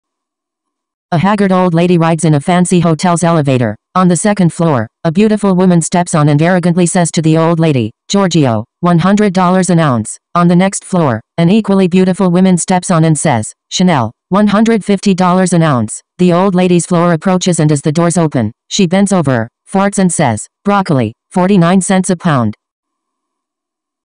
Jokes - A haggard old lady rides in a fancy hotel's elevator. On the second floor, a beautiful woman
Joke :
A haggard old lady rides in a fancy hotel's elevator. On the second floor, a beautiful woman steps on and arrogantly says to the old lady, "Georgio, $100 an ounce."
On the next floor, an equally beautiful women steps on and says, "Chanel, $150 an ounce."
The old lady's floor approaches and as the doors open, she bends over, farts and says, "Broccoli, 49 cents a pound."